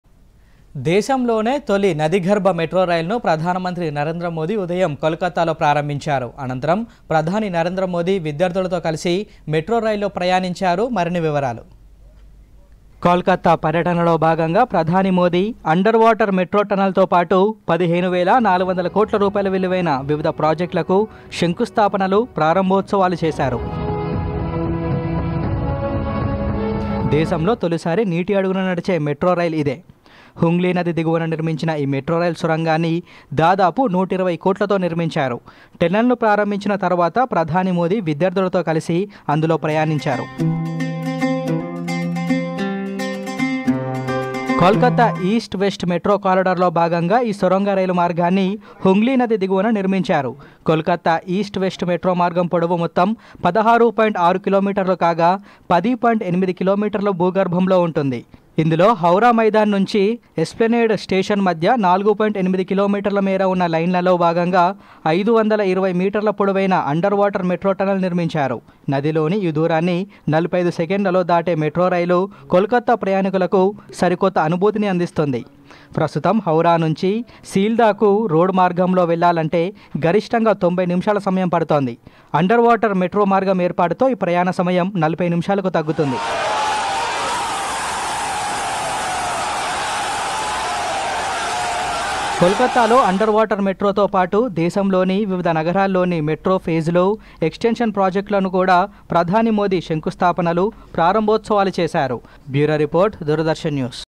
దేశంలోనే తొలి నదీగర్భ మెట్రో రైలును ప్రధాని మోదీ ప్రారంభించడంపై దూరదర్శన్ ప్రత్యేక కథనం
దేశంలోనే తొలి నదీ గర్భ మెట్రో రైలును ప్రధానమంత్రి నరేంద్రమోదీ ఈ ఉదయం కోల్ కతాలో ప్రారంభించారు. అనంతరం ప్రధాని నరేంద్రమోదీ విద్యార్థులతో కలిసి మెట్రో రైల్లో ప్రయాణించారు.
కోల్‌కతా పర్యటనలో భాగంగా ప్రధాని మోదీ.. అండర్‌ వాటర్‌ మెట్రో టన్నెల్‌తో పాటు 15 వేల  400 కోట్ల రూపాయల విలువైన వివిధ ప్రాజెక్టులకు శంకుస్థాపనలు, ప్రారంభోత్సవాలు చేశారు. దేశంలో తొలిసారి నీటి అడుగున నడిచే మెట్రో రైలు ఇదే. హుగ్లీ నది దిగువన నిర్మించిన ఈ మెట్రో రైలు సొరంగాన్ని దాదాపు 120 కోట్లతో నిర్మించారు. టన్నెల్‌ను ప్రారంభించిన తర్వాత ప్రధాని మోదీ విద్యార్థులతో కలిసి అందులో ప్రయాణించారు.
కోల్‌కతా ఈస్ట్‌ - వెస్ట్‌ మెట్రో కారిడార్‌లో భాగంగా ఈ సొరంగ రైలు మార్గాన్ని హుగ్లీ నది దిగువన నిర్మించారు. కోల్‌కతా ఈస్ట్‌ - వెస్ట్‌ మెట్రో మార్గం పొడవు మొత్తం 16.6 కిలోమీటర్లు కాగా.. 10.8 కి.మీ. భూగర్భంలో ఉంటుంది. ఇందులో  హావ్‌డా మైదాన్‌ నుంచి ఎస్‌ప్లెనెడ్‌ స్టేషన్ల మధ్య 4.8 కి.మీ.ల మేర ఉన్న లైనులో భాగంగా 520 మీటర్ల పొడవైన అండర్‌వాటర్‌ మెట్రో టన్నెల్‌ నిర్మించారు. నదిలోని ఈ దూరాన్ని 45 సెకన్లలో దాటే మెట్రోరైలు..కోల్‌కతా ప్రయాణికులకు సరికొత్త అనుభూతిని అందిస్తుంది. ప్రస్తుతం హావ్‌డా నుంచి సీల్దాకు రోడ్డు మార్గంలో వెళ్లాలంటే గరిష్ఠంగా 90 నిమిషాల సమయం పడుతోంది. అండర్‌వాటర్‌ మెట్రో మార్గం ఏర్పాటుతో ఈ ప్రయాణ సమయం 40 నిమిషాలకు తగ్గుతుంది.
కోల్‌కతాలో అండర్‌వాటర్‌ మెట్రోతో పాటు దేశంలోని వివిధ నగరాల్లోని మెట్రో ఫేజ్‌లు,ఎక్స్‌టెన్షన్‌ ప్రాజెక్ట్‌ల కూడా ప్రధాని శంకుస్థాపనలు, ప్రారంభోత్సవాలు చేశారు. ప్రారంభోత్సవ కార్యక్రమం అనంతరం పశ్చిమ బెంగాల్‌లోని నార్త్ 24 పరగణాల జిల్లా బరాసత్‌లో జరిగే బహిరంగ సభలో ప్రధాని ప్రసంగించారు.